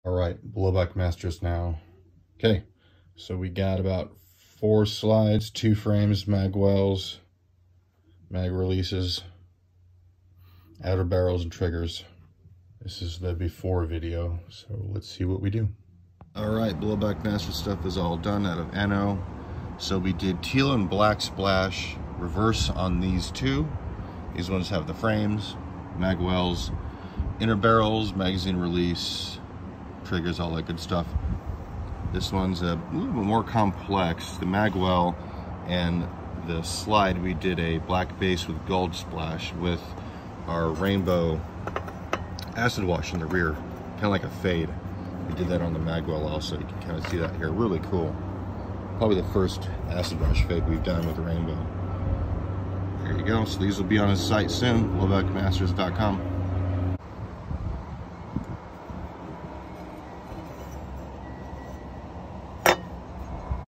Hi Capa airsoft custom anodized builds for Blowbackmasters before and after
Before and after footage of some HI Capa 4.3 and 5.1 build parts for Ben at blowback masters.